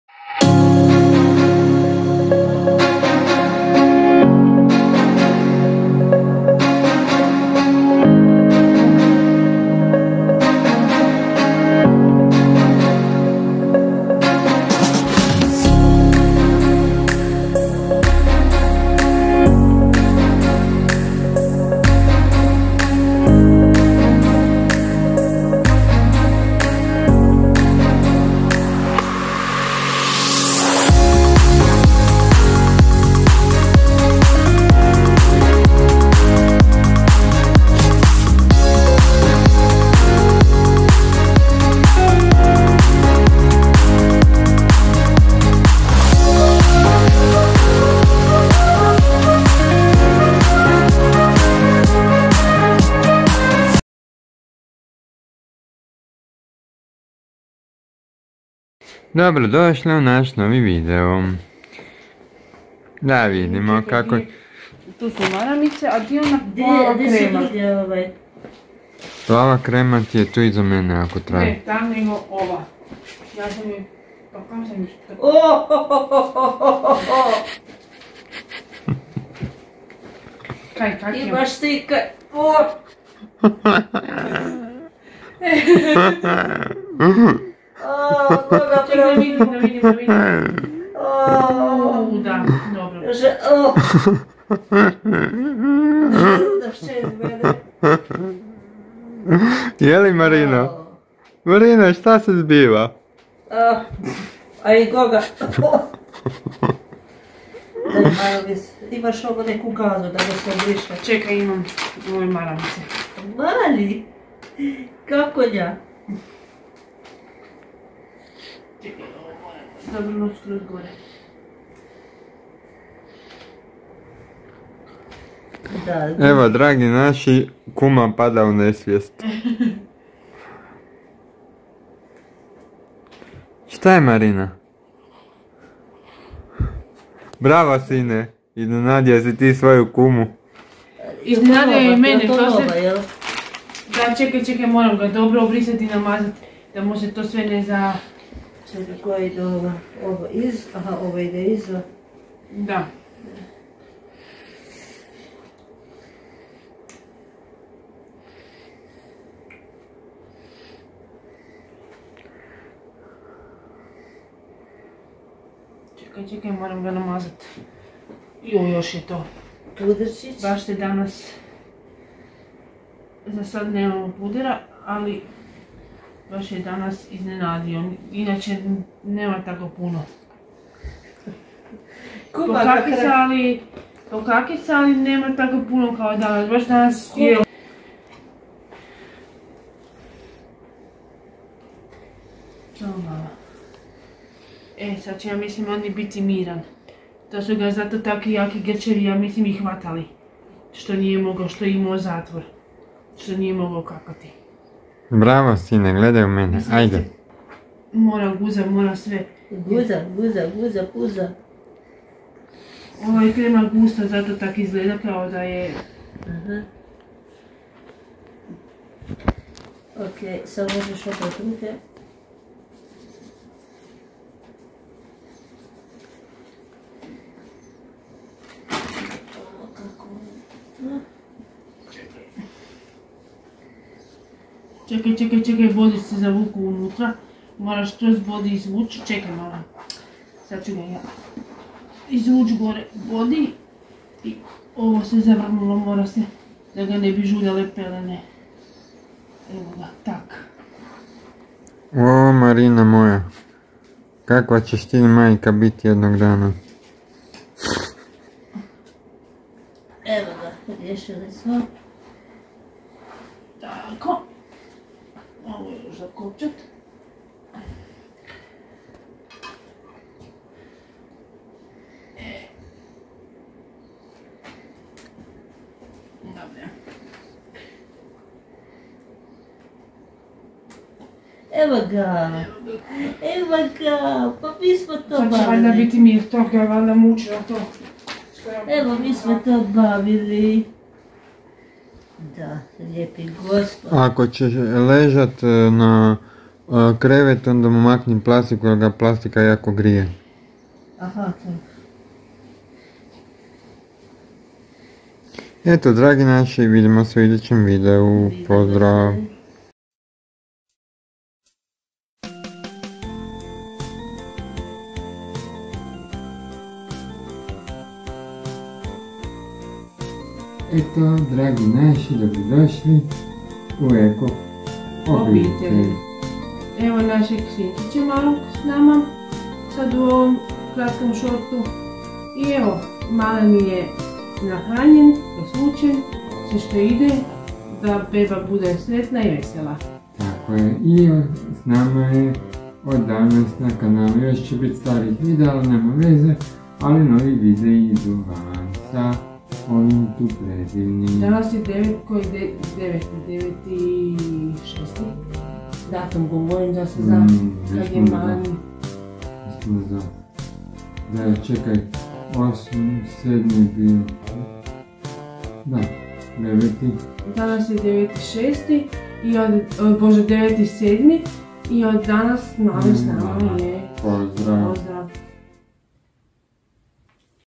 Moja supruga i Marina presvlače našeg Sina Ivana
Moja supruga je presvlačila Ivana zajedno s Marinom ali se marina skoro onesvijestila kada je moja supruga počela skidati pelenu
e draga Marina i ti čes jednog dana prolazit isto ko mi sada sve je to prirodno ali ako se voli svoje dijete ona to se radi s velikim zadovoljstvom i ljubavi